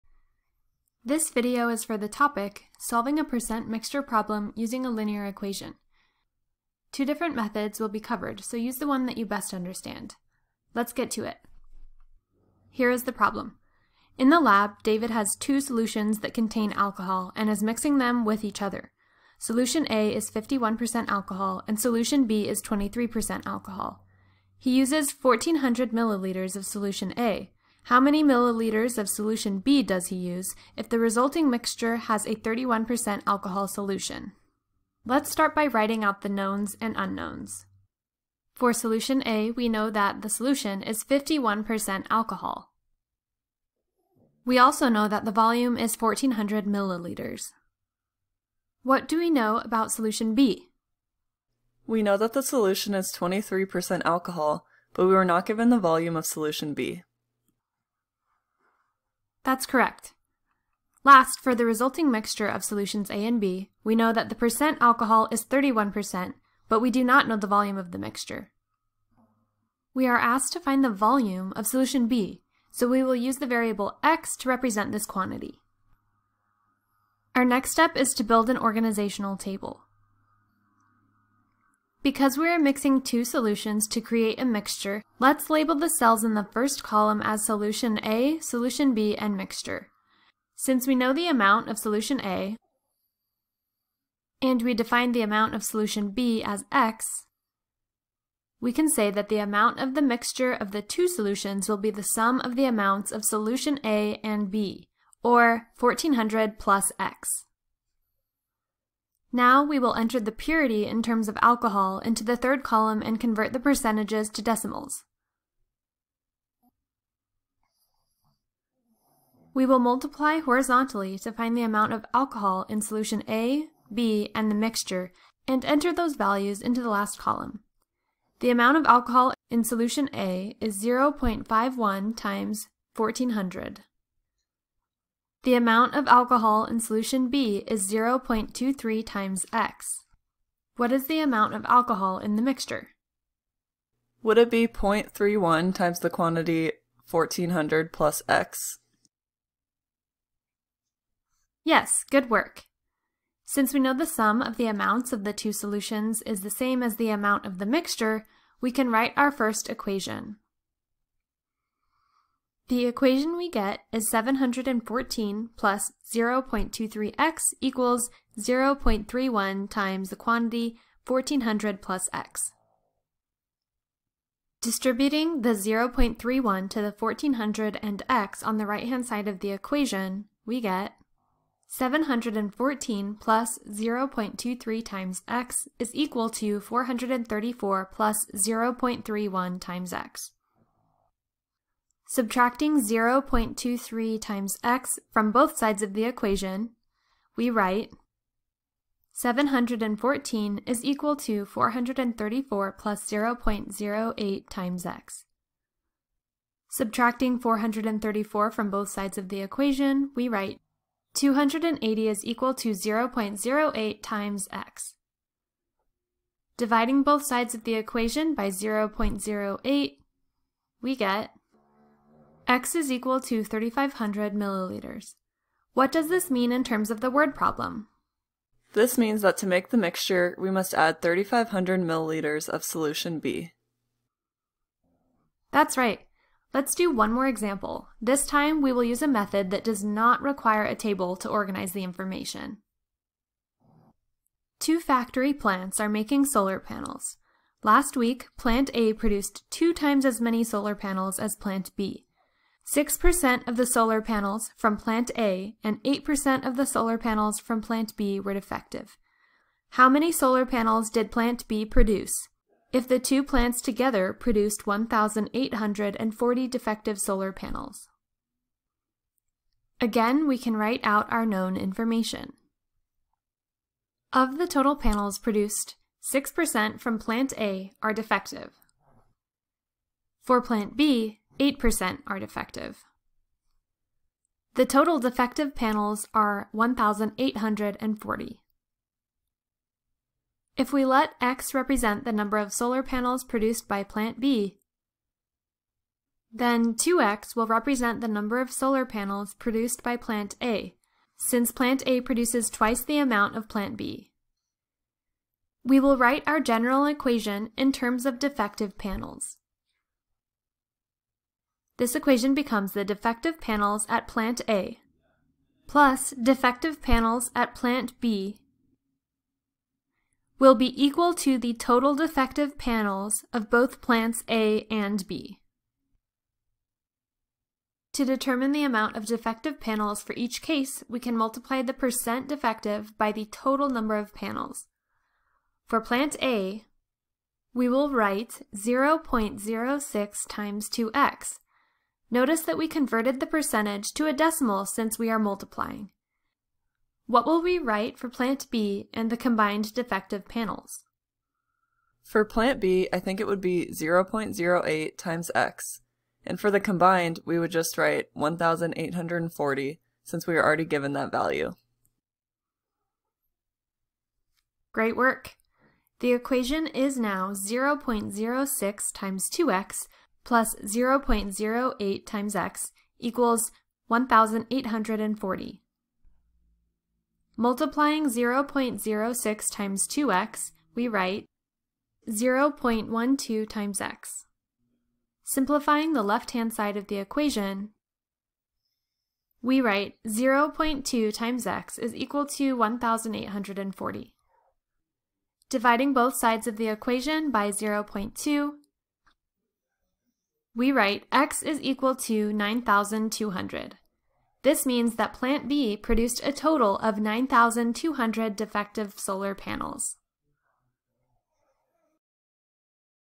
Solving a percent mixture problem using a linear equation
This is a video of Solving a percent mixture problem using a linear equation for Arizona State University.

Join us!